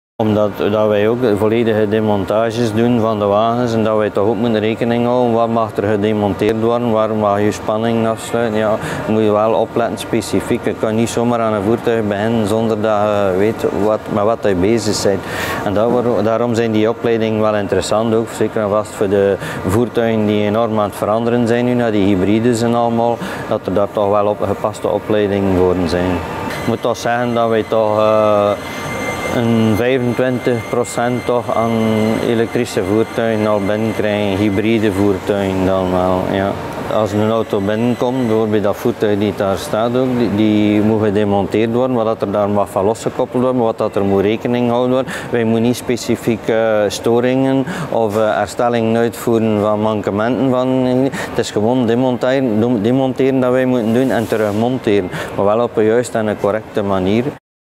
EDUCAM Sectorale HEV-certificering "veiligheid" (niv.1) - Ludwig Vromman
Waarom het belangrijk is om goed opgeleid te zijn om aan HEV voertuigen te werken.